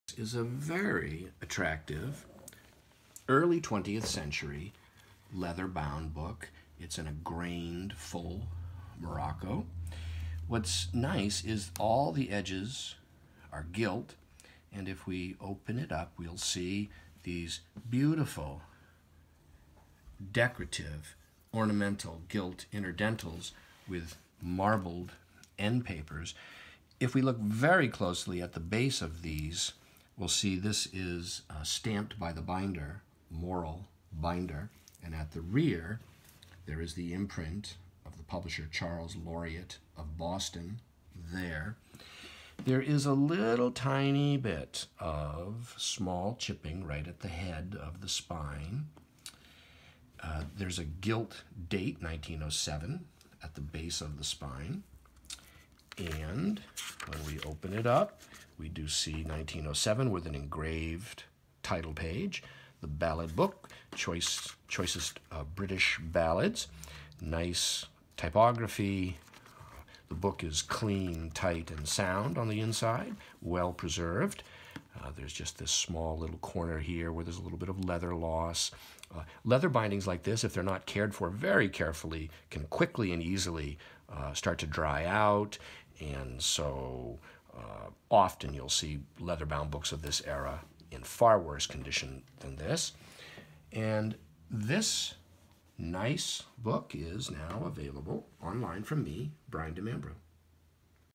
Ballad Book Allingham 1907 Morrell leather binding ornate gilt work lovely
Leather bound book, early 20th century, binding signed by Morrell Binder, Boston, Mass.

Grained full morocco, raised bands, ornate decorative dentelles, marbled endpapers, all edges gilt, engraved pictorial title page.

Leather remains supple,tiny chipping at spine head, slight loss at top corner of rear cover, all shown in video. Remains a beautiful book despite any age flaws.

Short video captures condition much better than words. Please watch prior to purchase to ensure a clear understanding of the item. Nicer than typical for the type.

The Ballad Book, A Selection of the Choicest British Ballads. Editied by William Allingham.
1907, London, MacMillan and Co., Limited.
pp. 393.

Book measures: 6 1/8" H X 4 1/4" W.

[V1085].